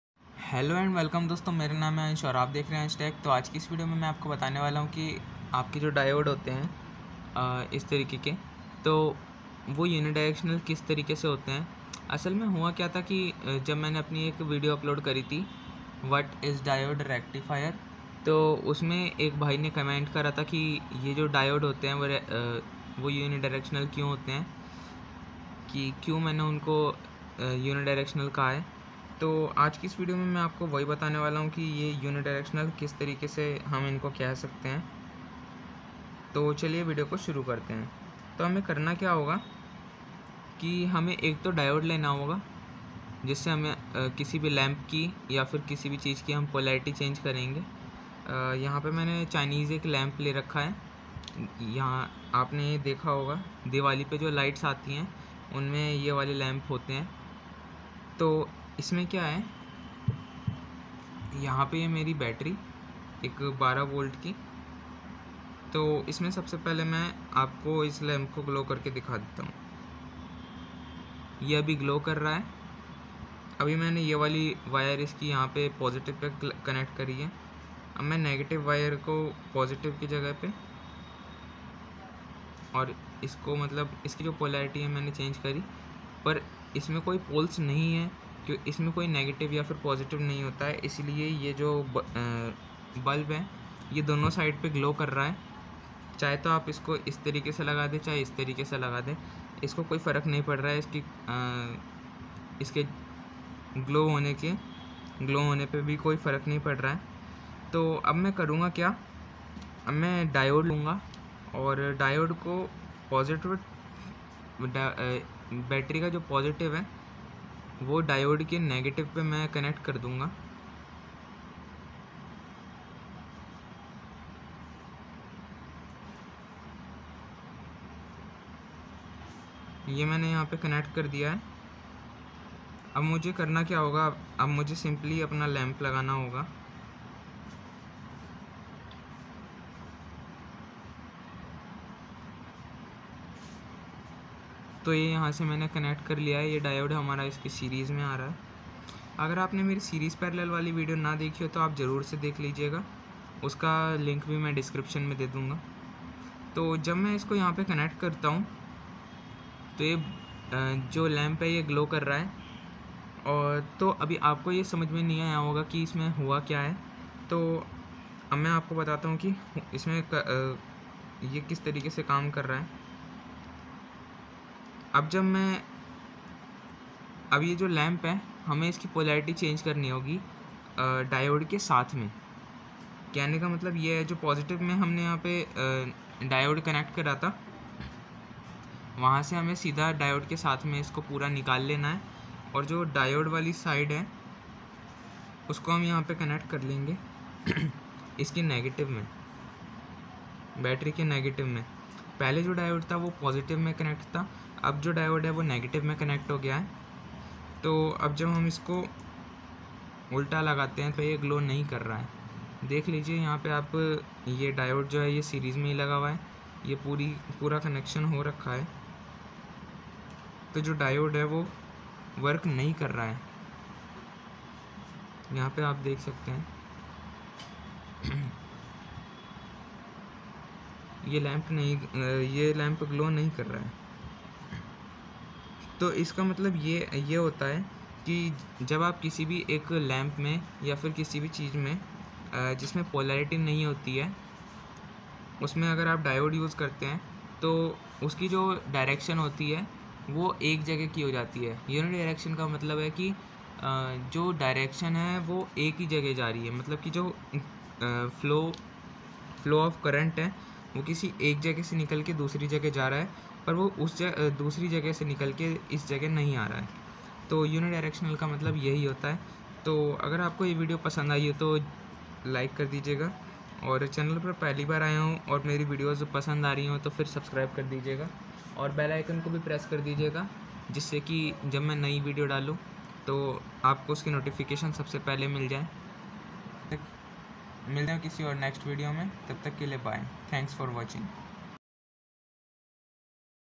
WHY DIODE IS UNIDIRECTIONAL EXPLAIN || IN HINDI || ~ÀNSH TECH
Hello friends, ajj ki video ka topic

WHY DIODE IS UNIDIRECTIONAL EXPLAIN || IN HINDI

In  this video i will talk about how the diode is unidirectional

Diode, unidiretinoal, bidirectional diode, diy, at home, how to make,

My all videos are related to TECH an circuits an you don't get any fake content on this channel.

                                                                                          ~ÀNSH TECH